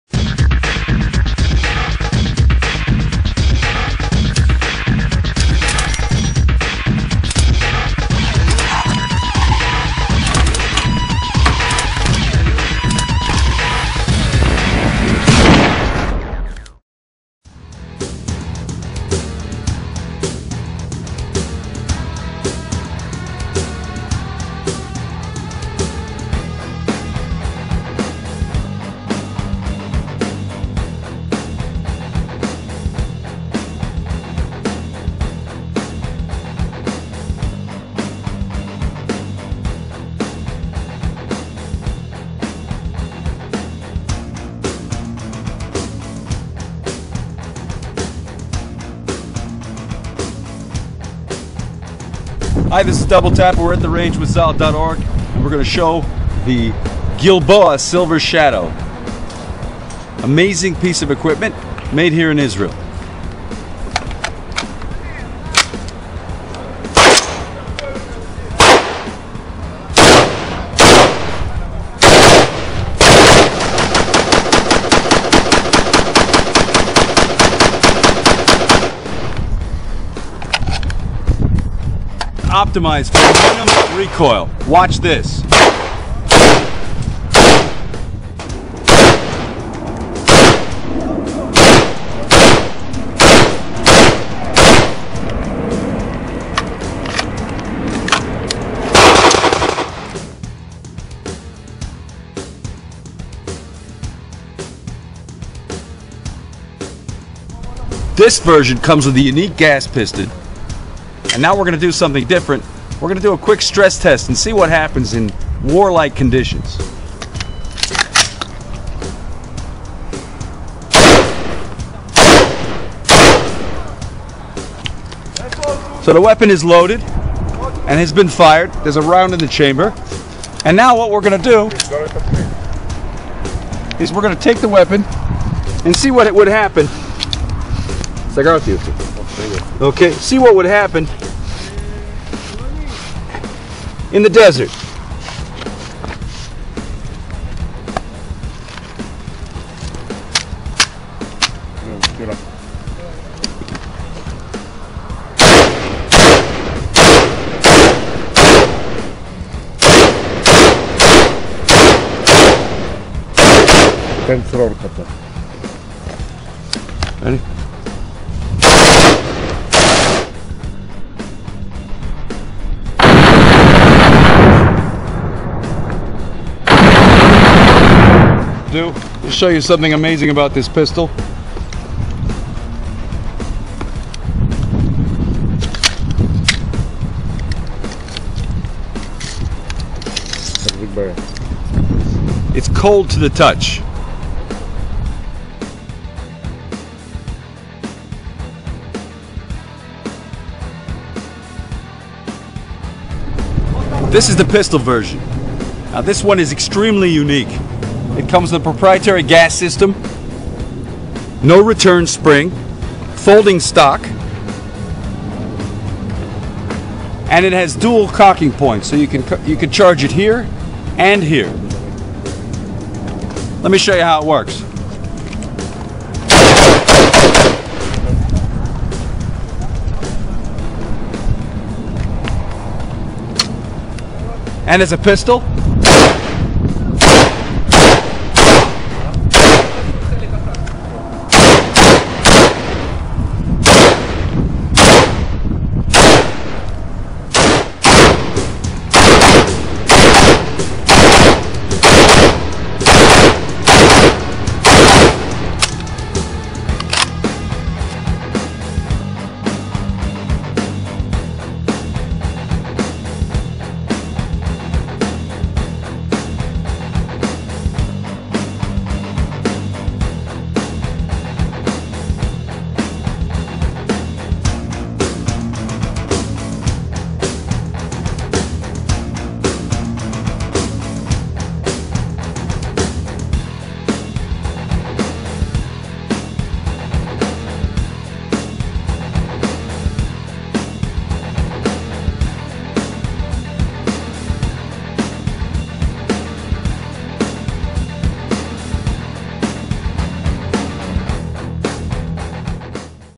ZAHAL - Gilboa Assault Rifle
The Gilboa™ line was specifically developed to provide a superior modern assault rifle. The Gilboa upper and lower receivers are fully machined from aircraft grade aluminum (7075) and hard anodized, resulting in a stronger weapon with no dimensional inconsistencies. Both structural strength and reliability have been improved by the redesigning of the proven platform. The Gilboa™ is manufactured using state of the art processes ensuring quality and dependability under the most extreme conditions. All Gilboa rifles are supplied with a military grade 1:7 chrome lined barrel unless otherwise requested. Gilboa rifles are typically manufactured in 5.56 NATO. Additional available calibers: 7.62x51, 7.62x39 and 9mm.